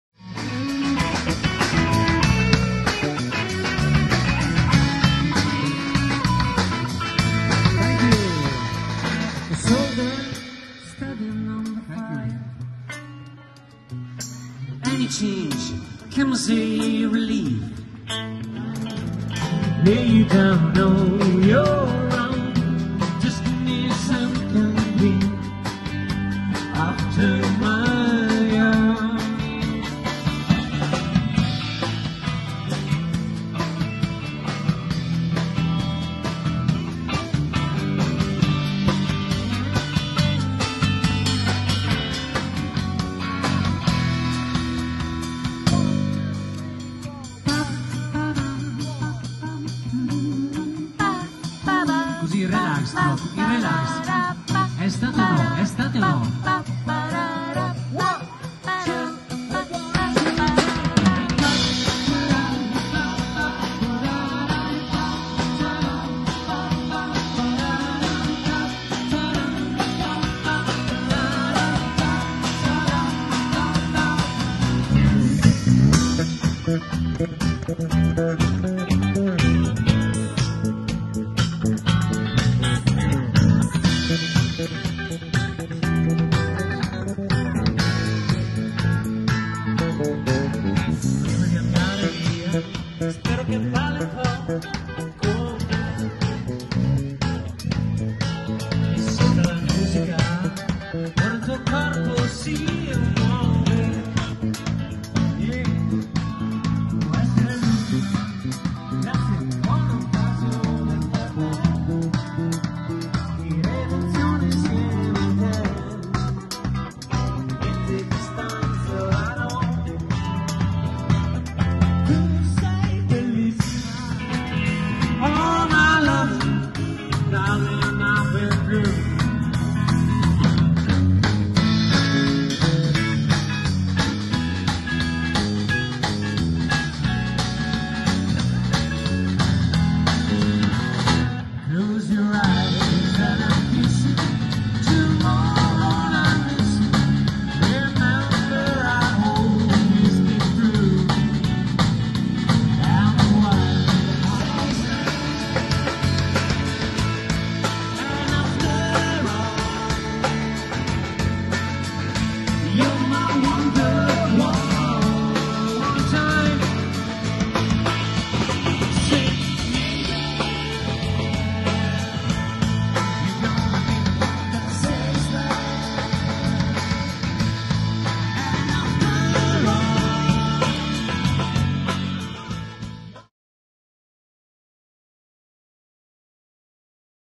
Pop Ems Mix Piazza Sirmione
Marco Gennari              Basso  e voce
Andrea Benedini           Chitarra e voce
Luca Modena               Batteria e voce